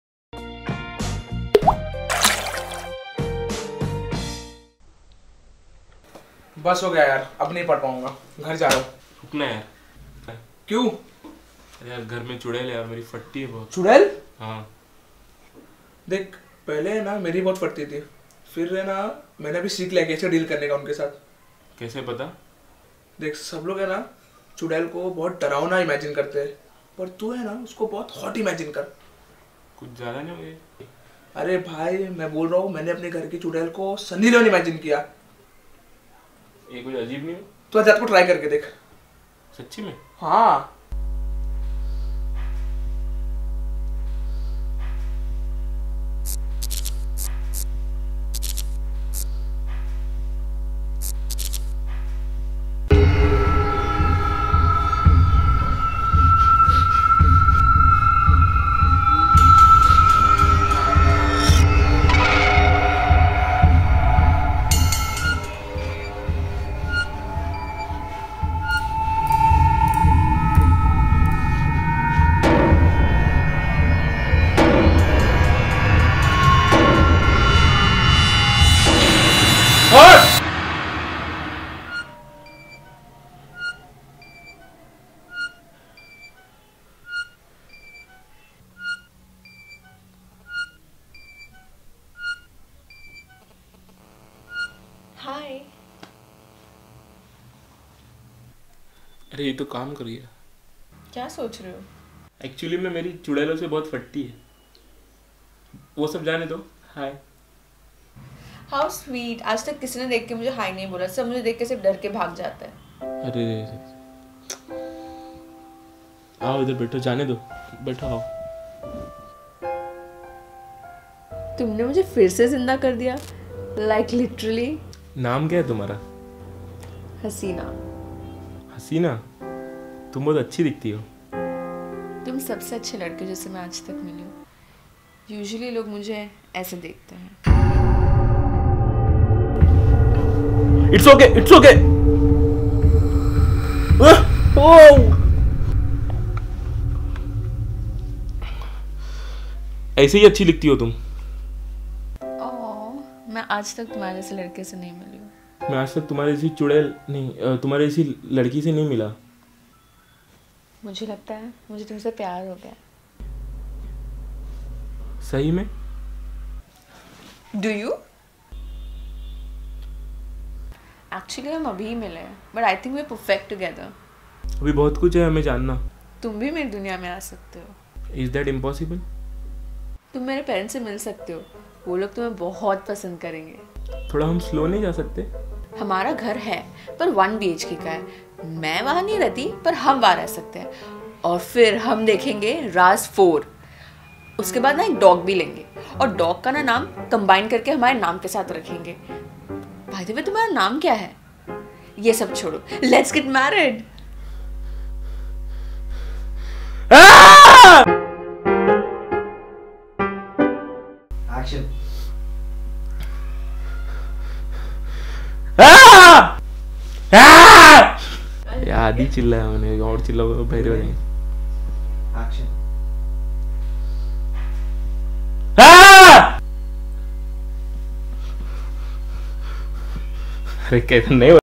When I Met Ghost Girlfriend - Valentine's Day Special - iDiOTUBE
Here is the latest "When I Met My Hot Ghost Girlfriend" sketch. Hope you guys like our attempt in doing something new for you all... Let us know your feedback in the comment section below. We would love to hear back from you all 😀🤘

Directed By : Mazin Mulla & Jay Singal .

Production By : Mazin Mulla & Jay Singal .

CAST:
Avishruti Mitra
Akash Shukla
Mazin Mulla